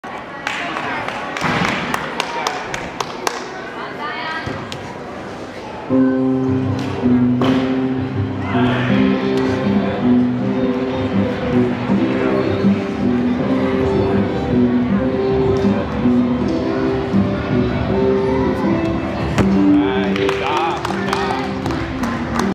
Charlotte Whitmark Bars Glider Invite 2024 Wildfire Gymnast Level 4
Bars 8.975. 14th AA 36.100.